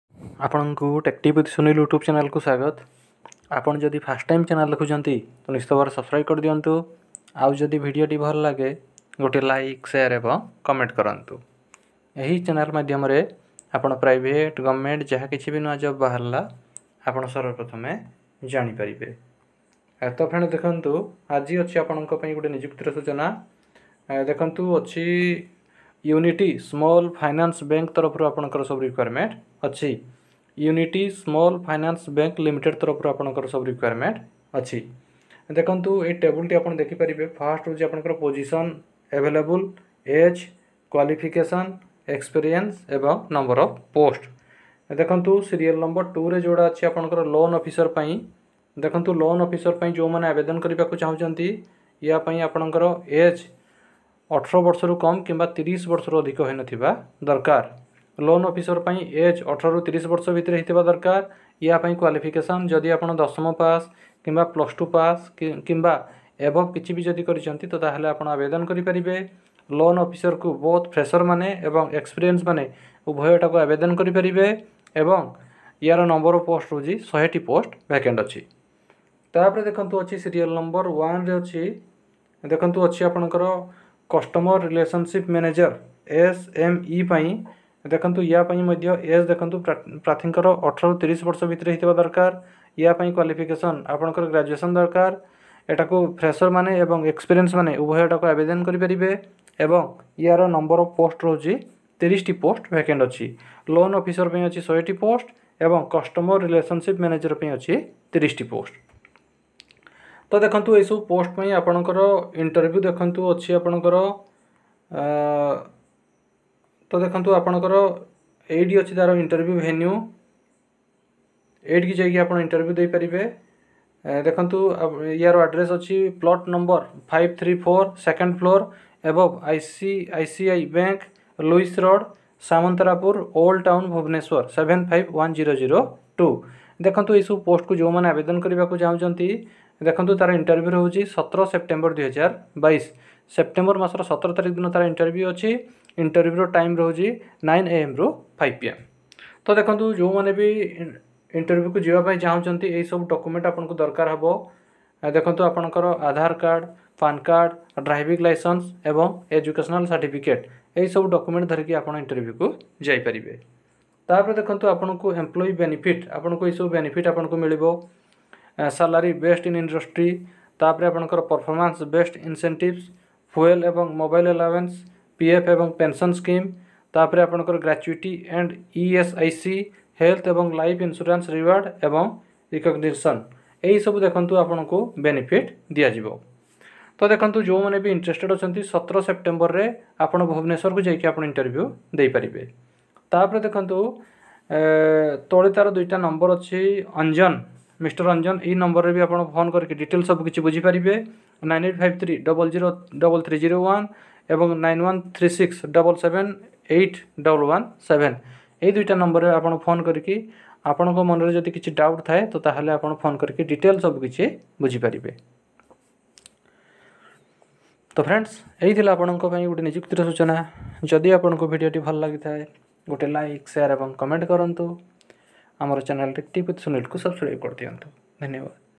USFBL bank recruitment 2022!! For various post!! Odisha latest job notification 2022!!
USFBL bank recruitment 2022.
For various post in all over odisha